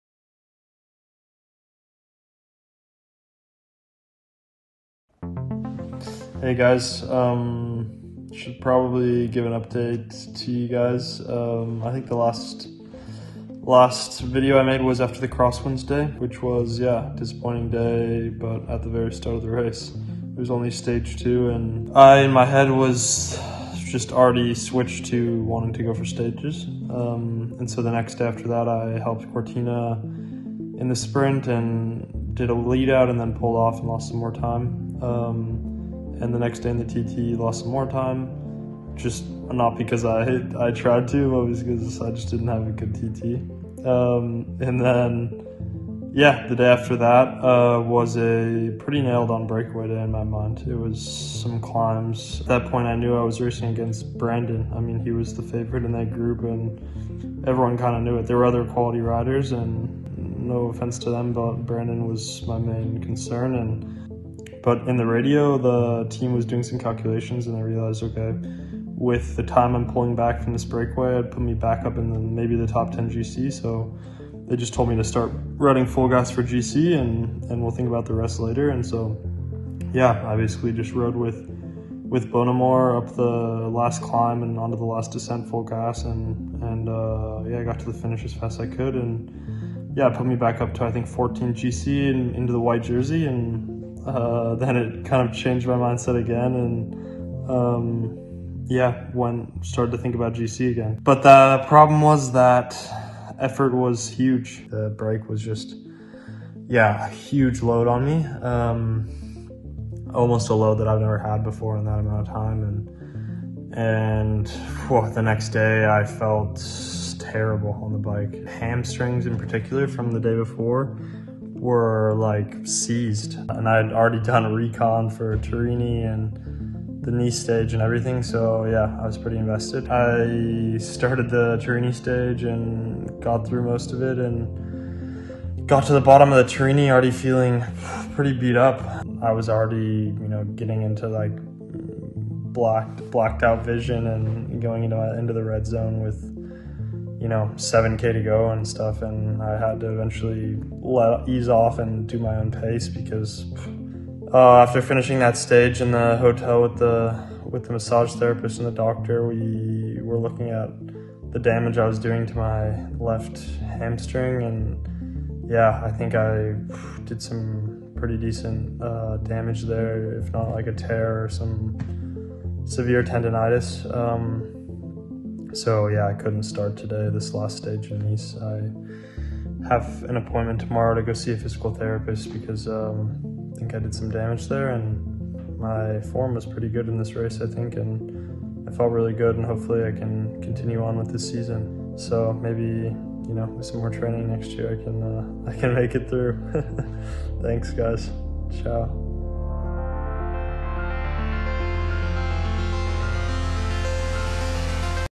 How Matteo Jorgenson Rebuilt His Paris-Nice, Just To Lose It All Again
Matteo Jorgenson of Movistar explains how he found himself back in the running in Paris-Nice after a crash, just to lose it all once again.